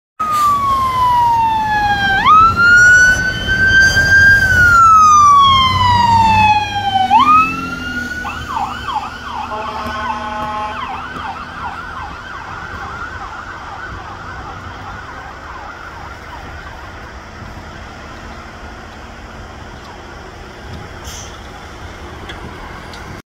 Hooksett NH Ambulance 2 responding to a medical back on 2/16/24.
Video by Steven Medeiros